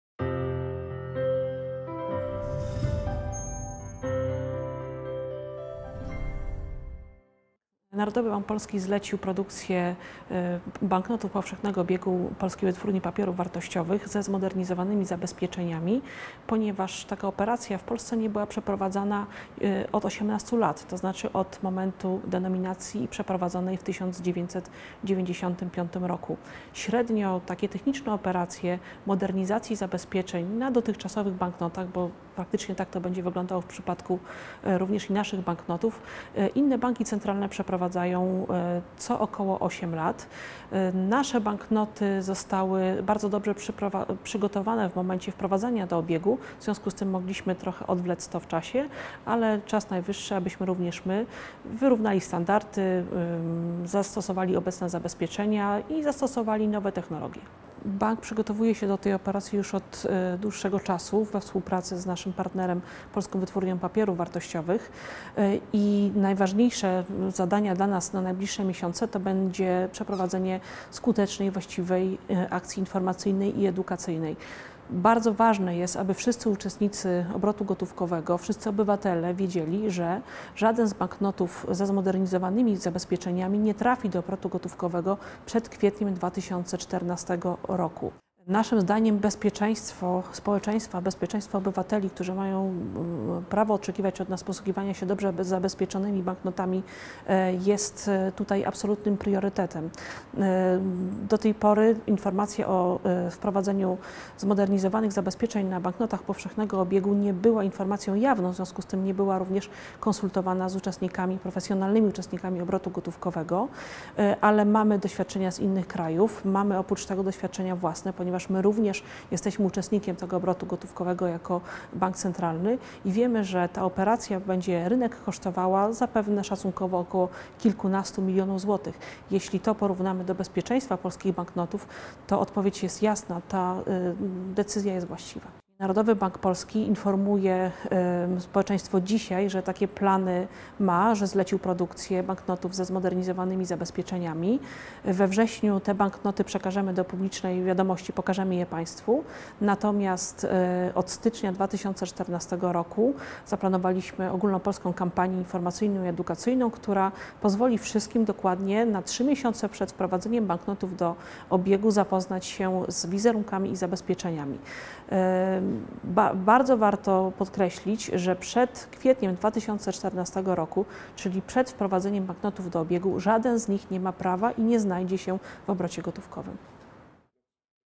Barbara Jaroszek o nowych zabezpieczeniach polskich banknotów
Narodowy Bank Polski zlecił Polskiej Wytwórni Papierów Wartościowych produkcję banknotów powszechnego obiegu ze zmodernizowanymi zabezpieczeniami. Projekty graficzne banknotów nie ulegną zmianie, a te z nowymi zabezpieczeniami trafią do obiegu w kwietniu 2014 roku.
Ze względów bezpieczeństwa szczegółowe informacje oraz banknoty z nowymi zabezpieczeniami zostaną zaprezentowane we wrześniu 2013 roku.